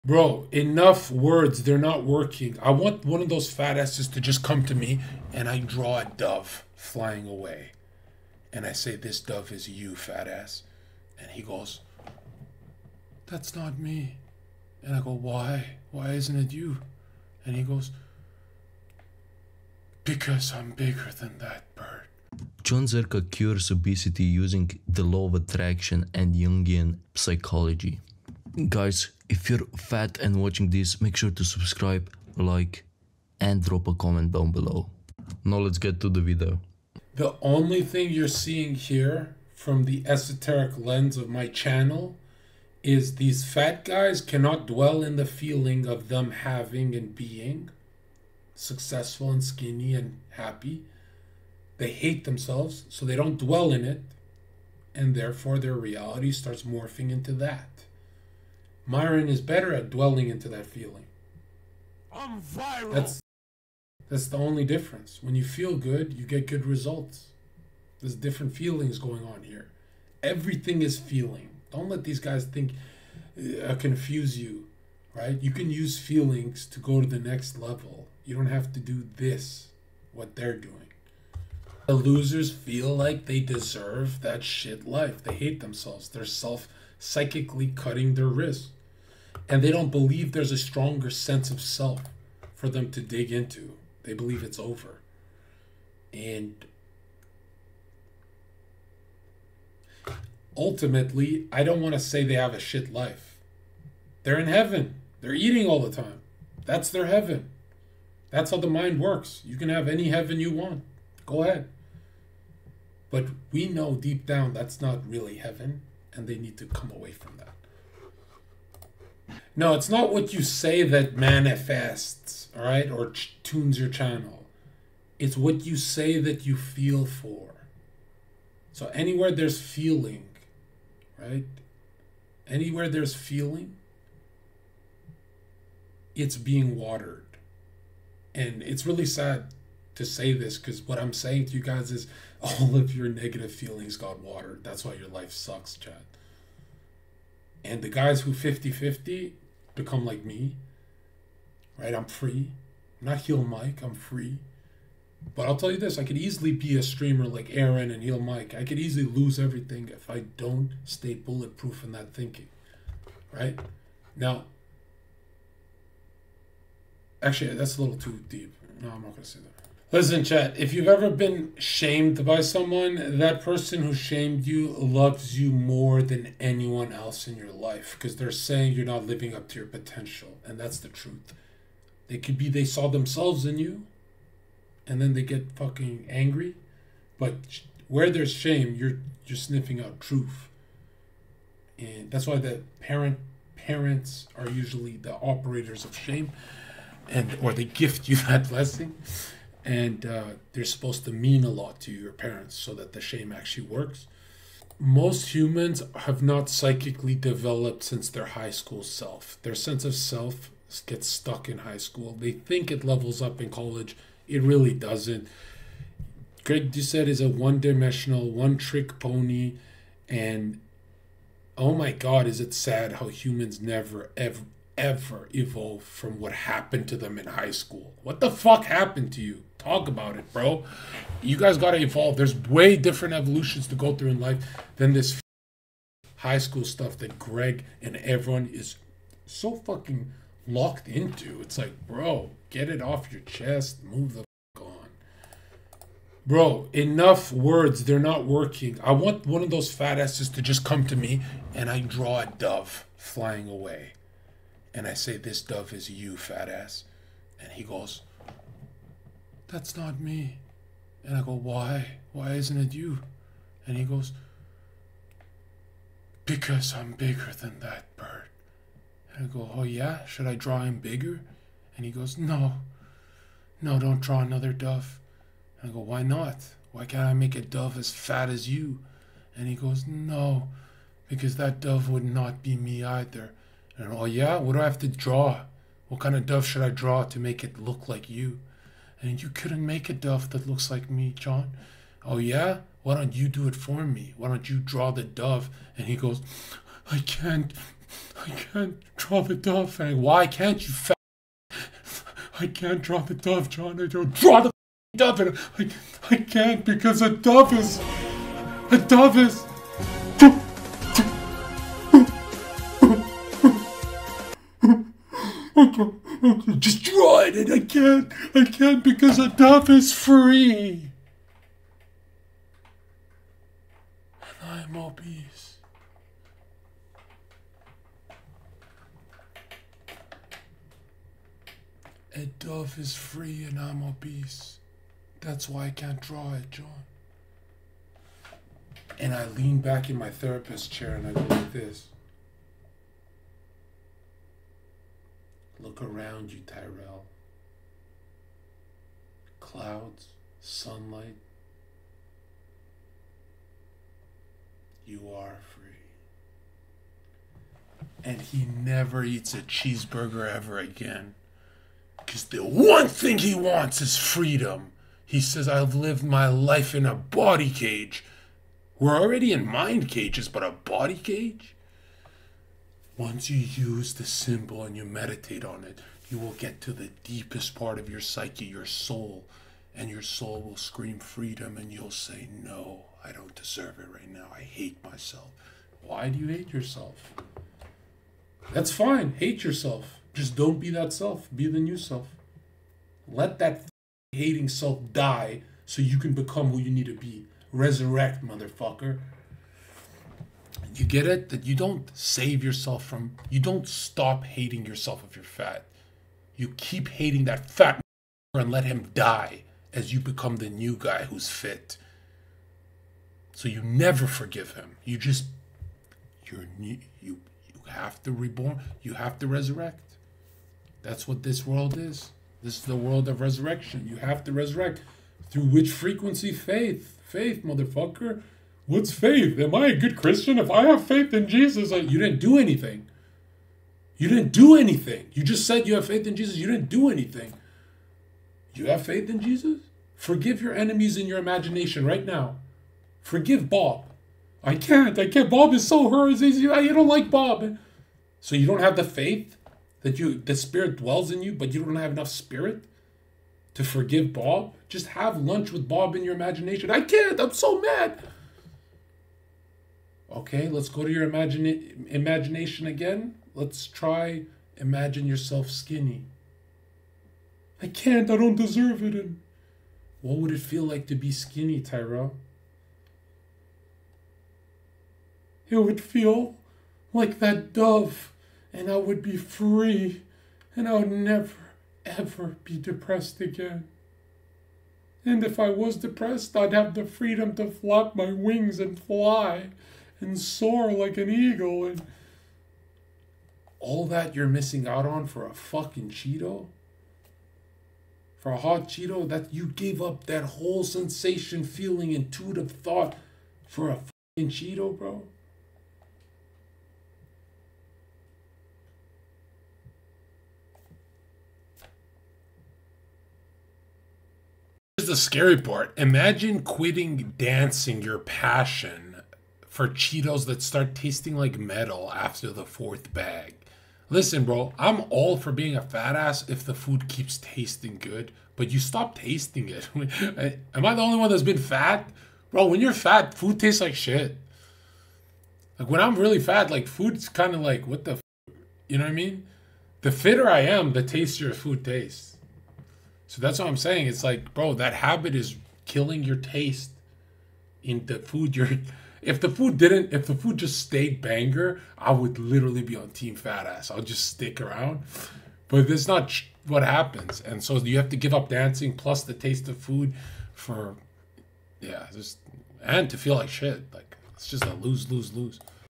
Zherka Cures Obesity Using Jungian Psychology
Video Editor:
Davinci Resolve